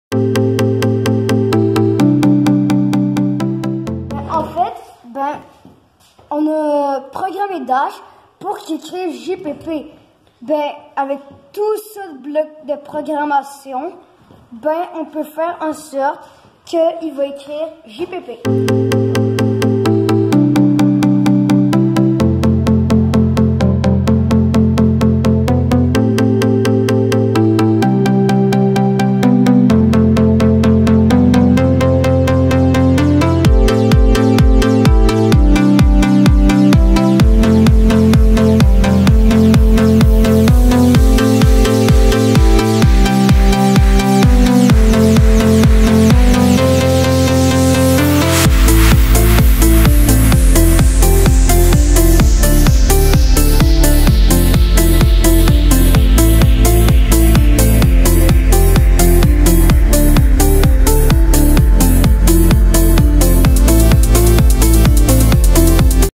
RoboTIC école NDA - 4e année
Pour souligner le Printemps numérique à Montréal, l’organisme Je Passe Partout lançait un concours de robotique et programmation auprès d’une centaine d’élèves des écoles primaires et secondaires partenaires.